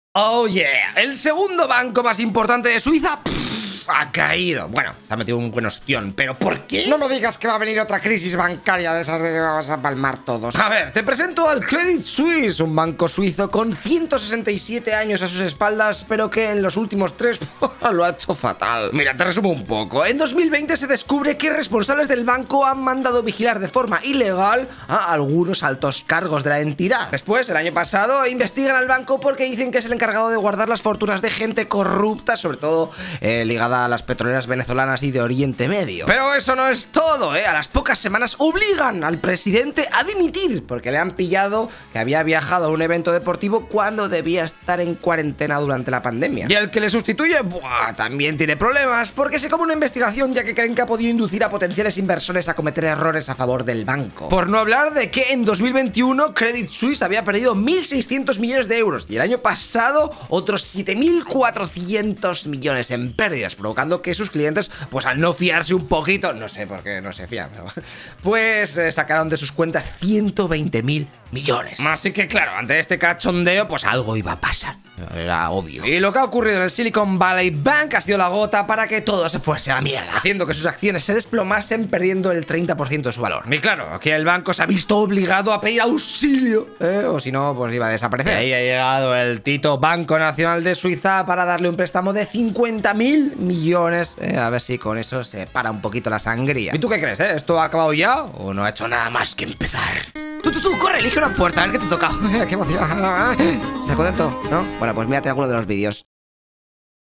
¿Qué le pasa a CREDIT SUISSE? - Nuevo Fail bancario
El segundo banco mas importante de Suiza, ha caido! Bueno, se ha metido un buen ostión, pero xq? Se viene otra crisis bancaria del horror.
A ver..  te presento al Credit Suisse, un banco con 167 años a sus espaldas, pero que en los ulitmos 3, lo ha hecho todo fatal.
En 2020 se descubre que responsables del banco han mandado vigilar de forma ilegar a algunos altos cargos de la entidad.
El año pasado investigan al banco porque dicen que es el encargado de guardar las fortunas de gnete corrupta, sobretodo ligada a las petroleras venezolanas y de Oriente Medio.
Pero eso no es todo, a las pocas semanas, obligan al presidente a diomitir xq le pillan que había viajado a un evento deportivo cuando debia de estar haciendo cuarentena durante la pandemia.
El que le sustiuye  tambien se come una investigación ya que creen que ha podido inducir a potenciales inversores a que cometan errores a favor del banco.
Por no hablar de que en 2021 el Credit Suisse había perdido 1.600 millones de euros y el año pasado otros 7.400, provocando que sus clientes al no fiarse sacasen de sus cuentas 123.000 millones.
ASi que ante este cachondeo… esta claro que iba a pasar algo. Y lo ocurrido con el Sillicon Valley Bank ha sido la gota para que todo se fuese a la merde. Sus acciones se han desplomado, perdiendo el 30% de su valor.
Viendose obligado a pedir auxilio o sino desaparecia… y ahí a llegado el Banco Nacional de Suiza para darle un préstamo de 50.000 millones a ver si con eso se para la sangria.
Tu que crees? Esto ha acabado ya o no ha hecho nada mas que empezar?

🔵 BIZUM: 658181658
🟡 Crypto (USDT) Red (TRC20): TLfmjfK11xKRVCzRJYgsCiHUwoDfWJvS5n

3º Únete a la comunidad ilustrada?